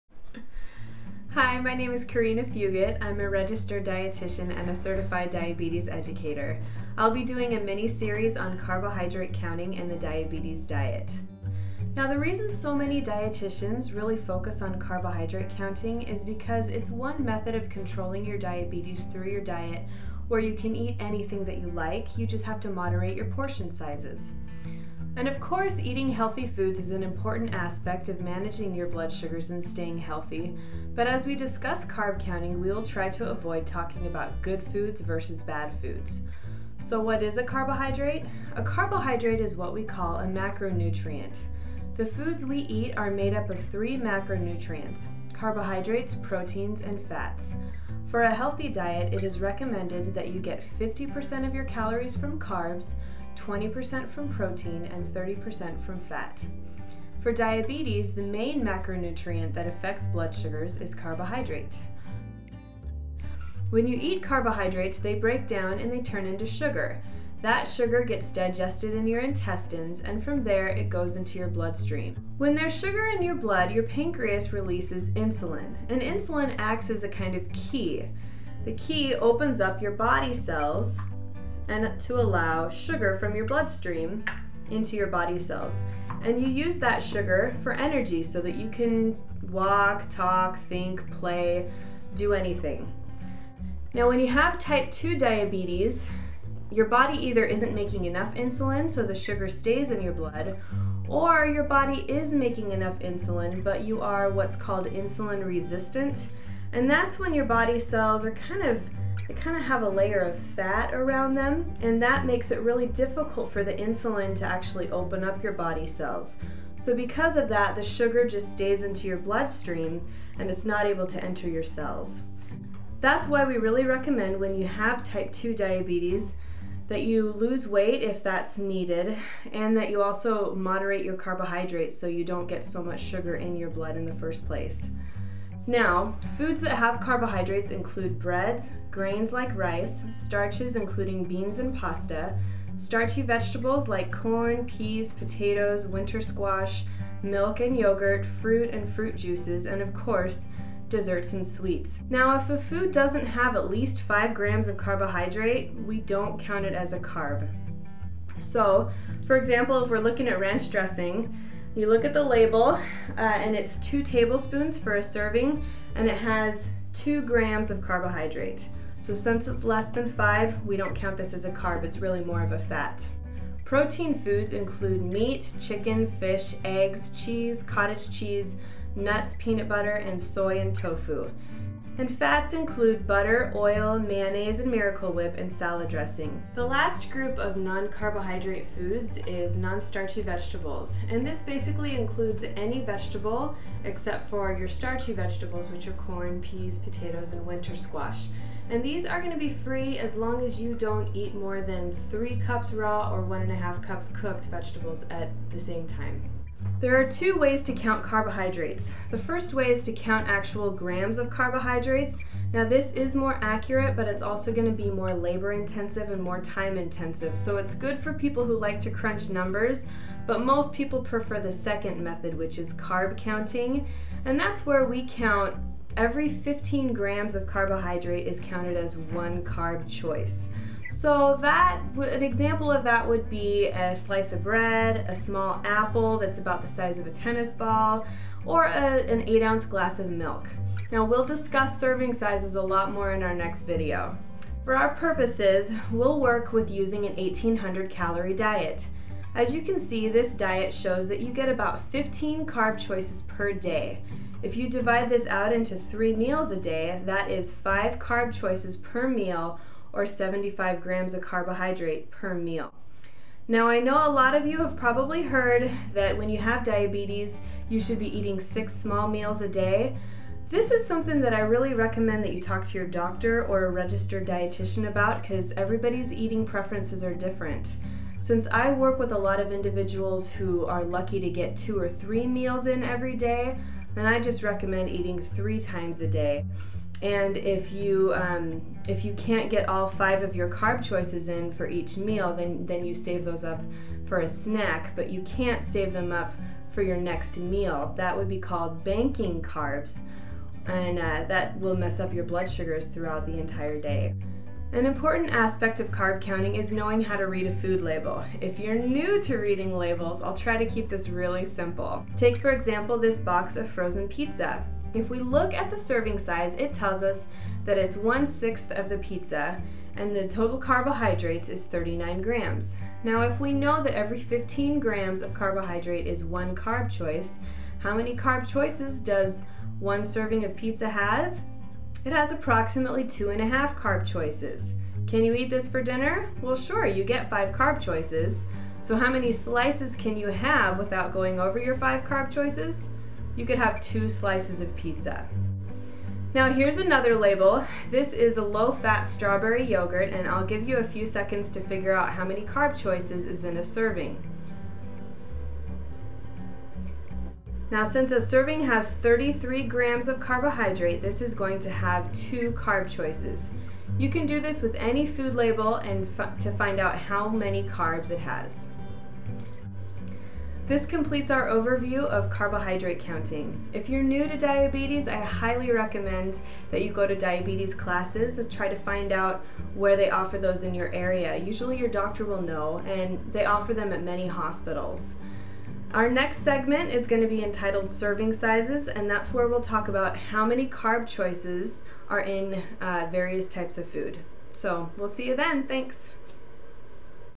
Diabetes Diet 1:5- Carbohydrate Counting
So you've got diabetes, or you know someone who has diabetes? You're probably wondering what the "diabetes diet" is all about, or how you need to change your eating habits in order to control your blood sugar, right?

This video is the first in a series on healthy eating for diabetes. Specifically, it is an overview on carbohydrate counting. It includes instructions on carb identification, healthy eating, label reading, and how carbs turn to blood sugar.

Presented by a Registered Dietitian and Certified Diabetes Educator who conducts one-on-one nutrition counseling and diabetes self-management education courses.